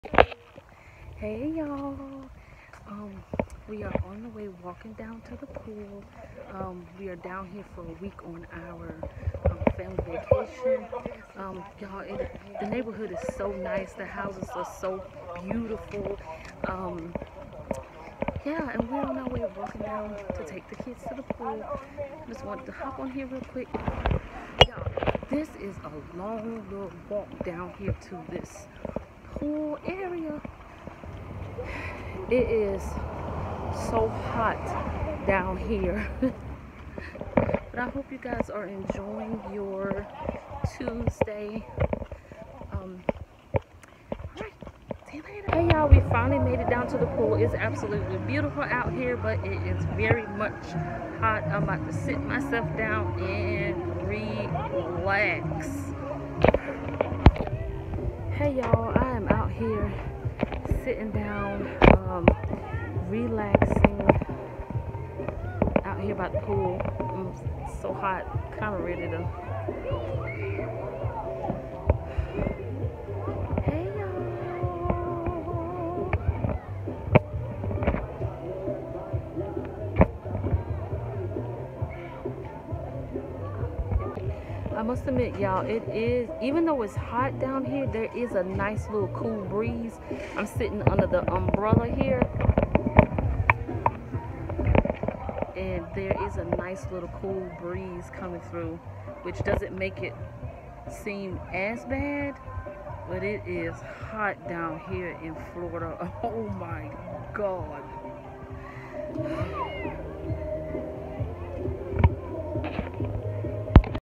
Dont miss out on my family's first-ever vlogging family vacation
Family Vacation time at the pool😊 It was so hot 🥵 but it was relaxing 😎